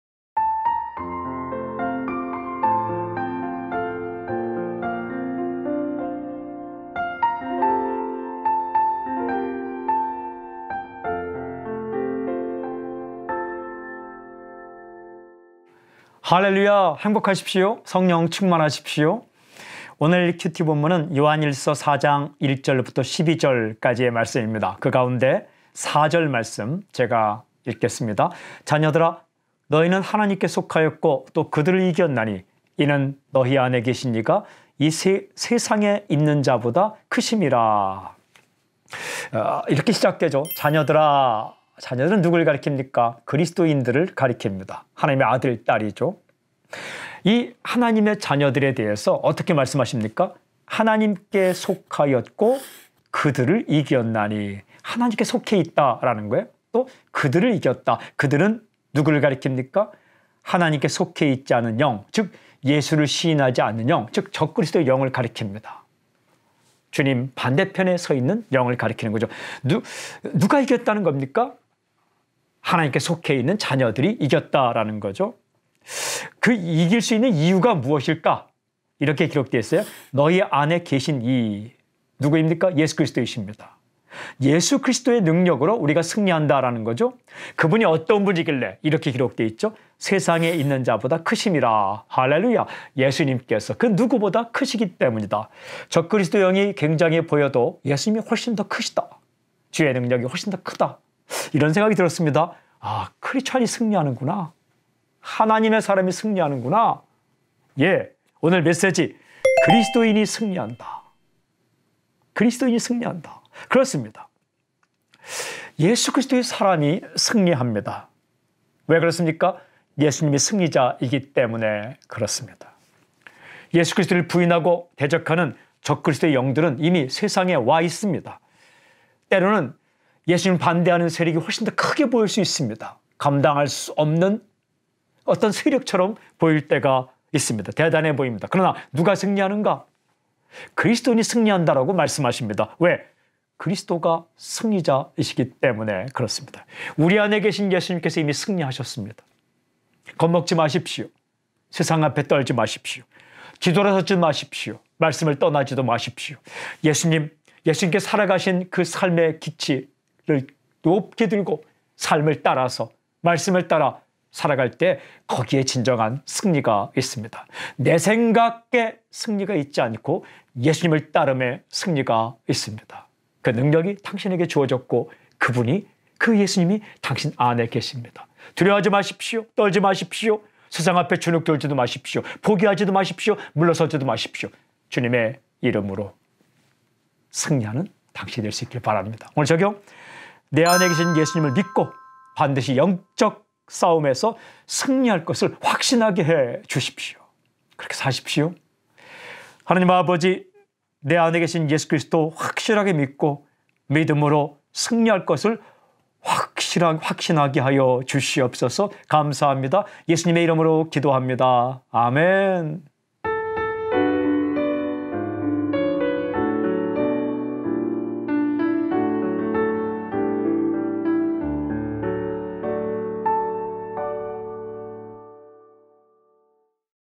2021.1.6 - 라준석목사의 원포인트 큐티메세지
원포인트 메세지: "그리스도인이 승리한다."
본문: 요한1서 4장 4절